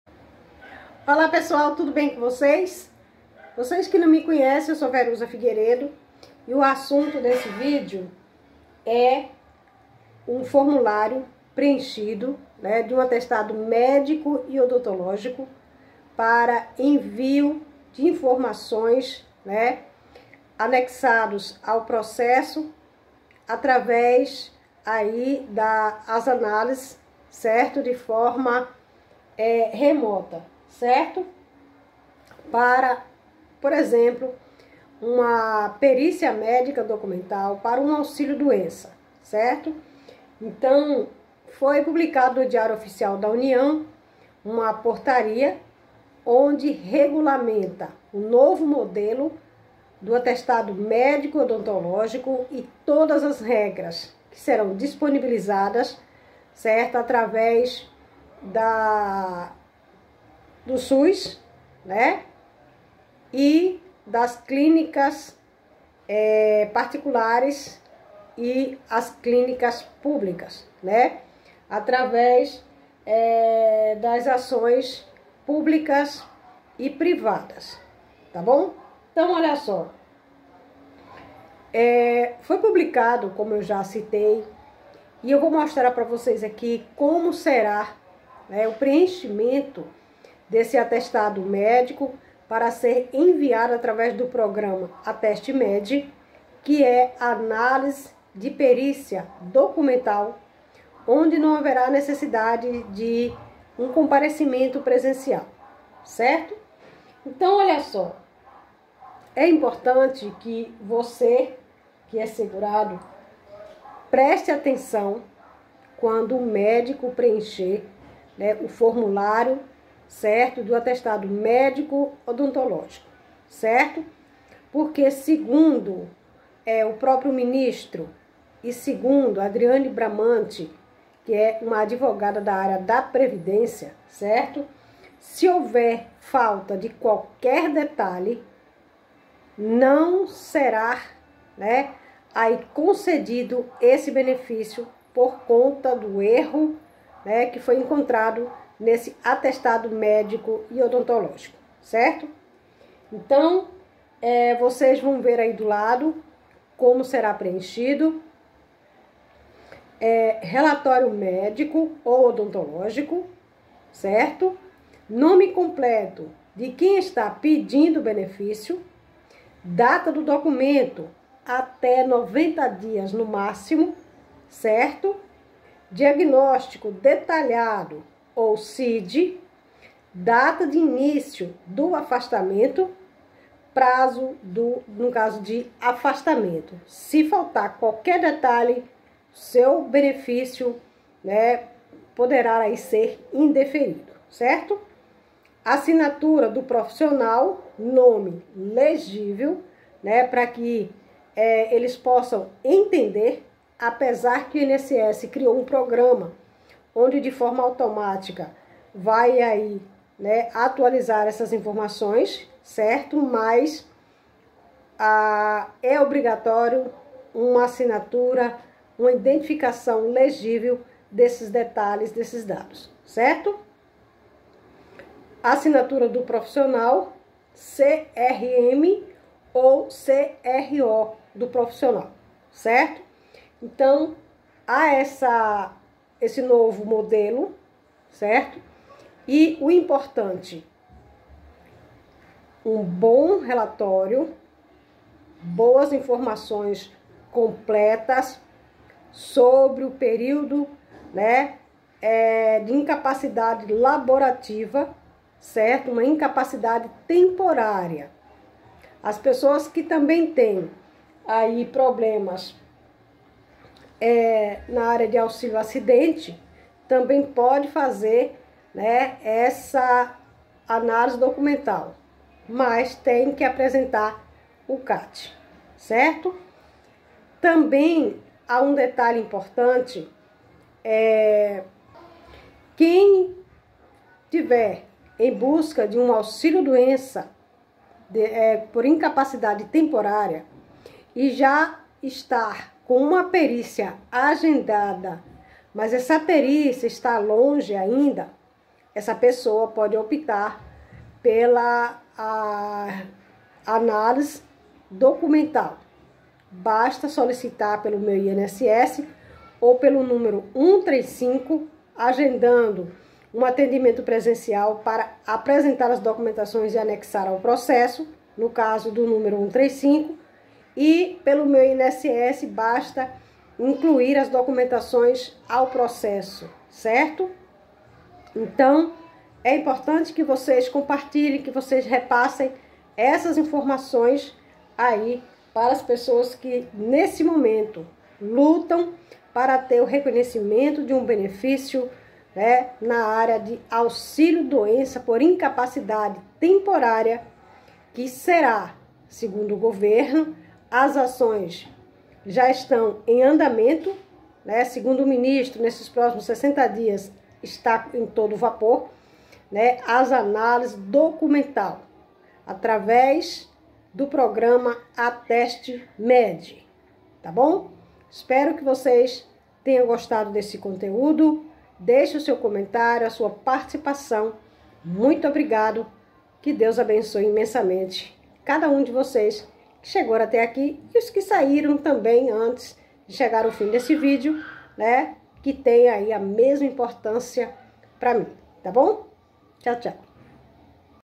Como preencher os dados no atestado médico/Odontológico
Atestado médico/Odontológico

Como será preenchido o formulário de um Atestado médico para enviar para uma perícia médica documental.